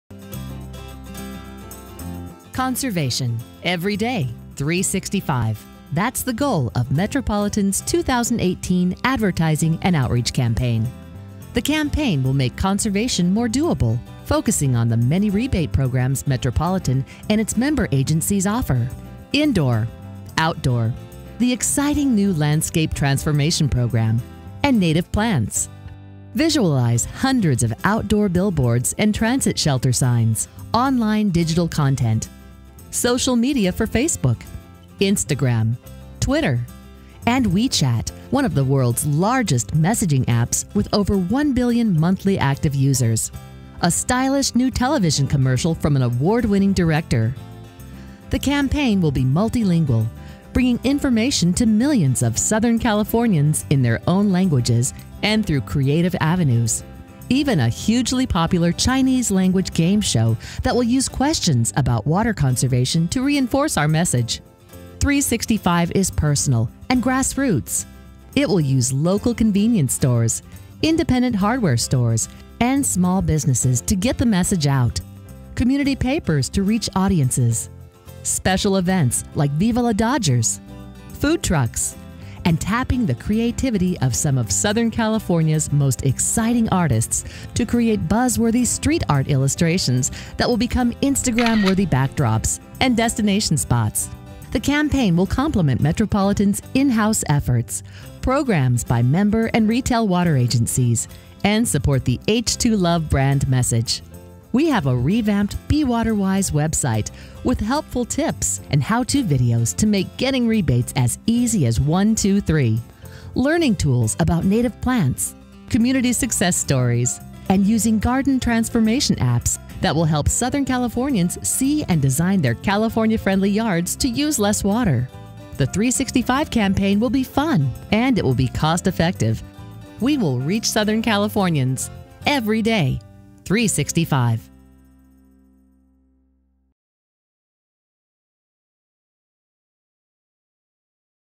Metropolitan's 2018 Conservation Campaign
An overview of the 365 every day conservation campaign.